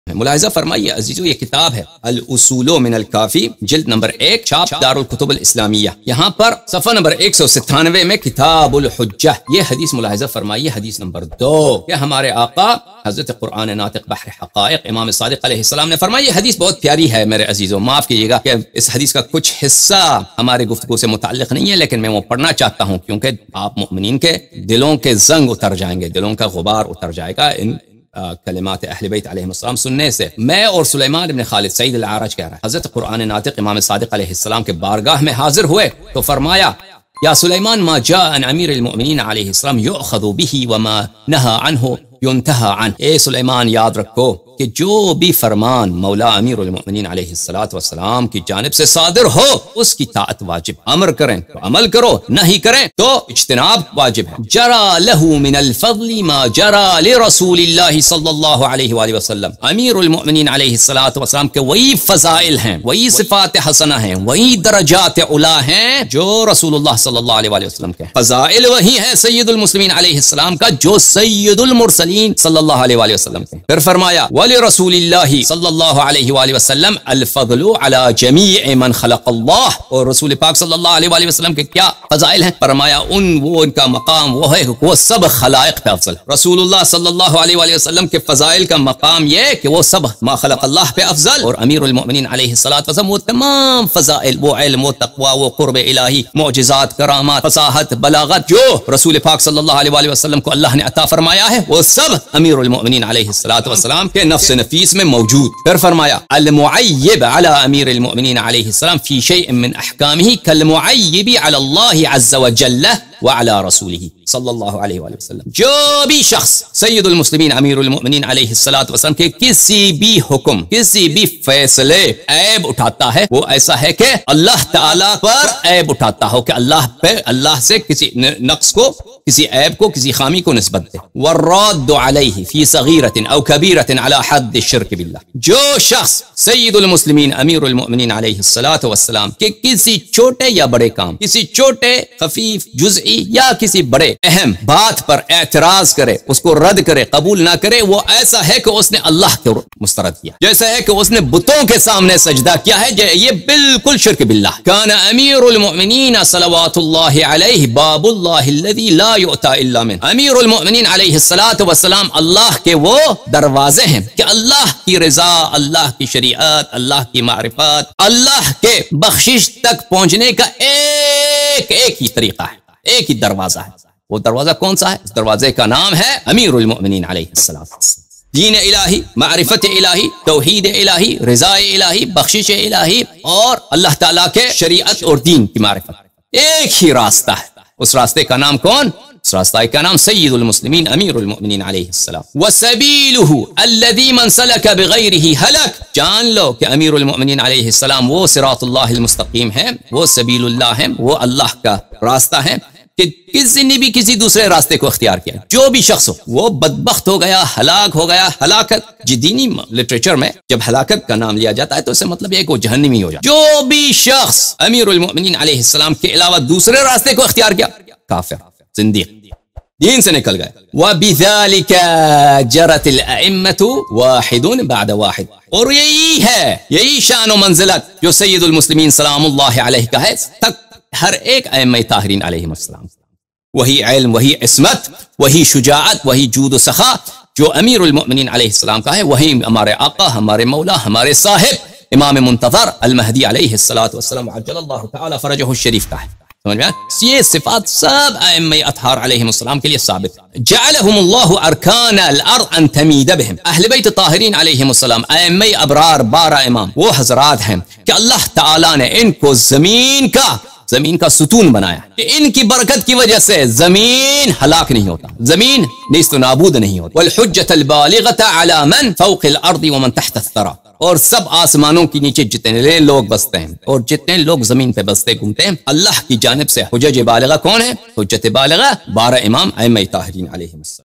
Maula Ali as Ki Yeh Haqeeqat Jaan Kar Bhai Agar Hidayat Nahi Milti Tou Tumhari Qabr O Qayamat | FH
Streamed live on 1 Nov 2021
احقاق الحق و ازهاق الباطلFor
Live Debate with Sheikh Hassan Allahyari